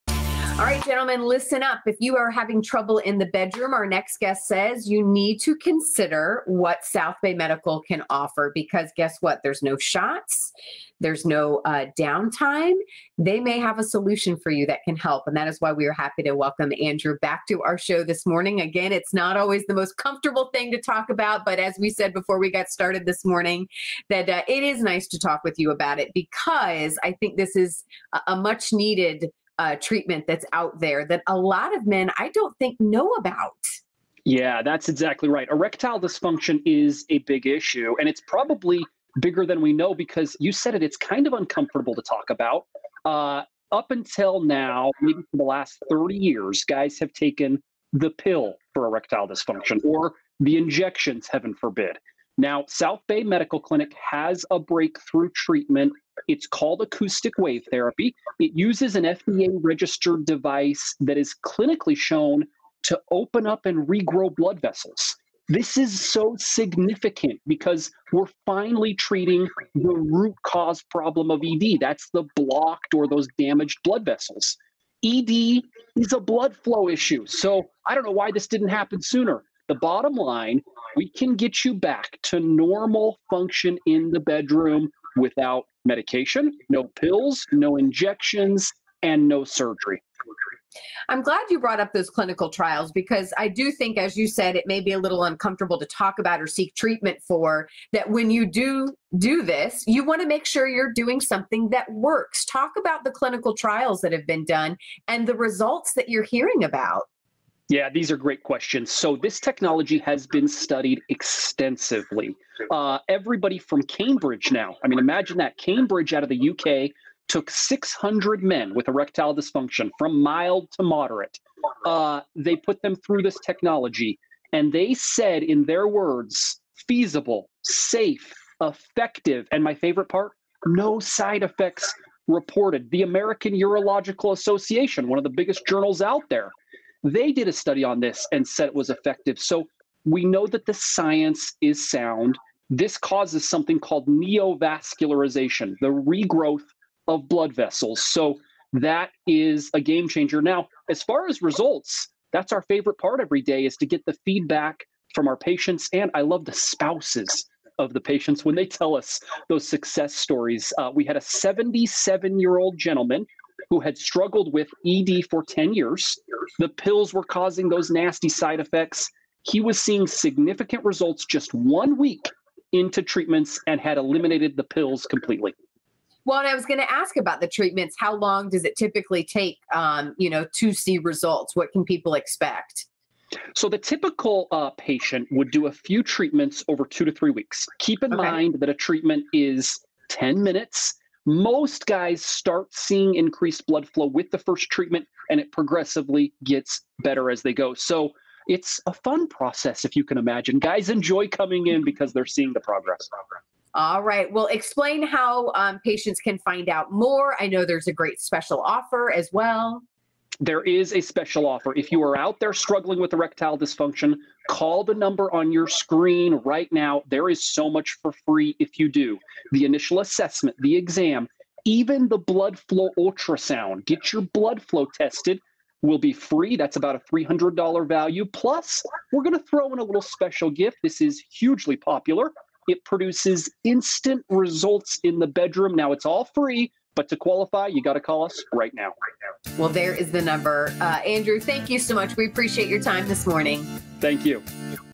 South Bay Medical | Morning Blend
South Bay Medical Clinic has a breakthrough treatment for ED that does not require a pill, injection or surgery!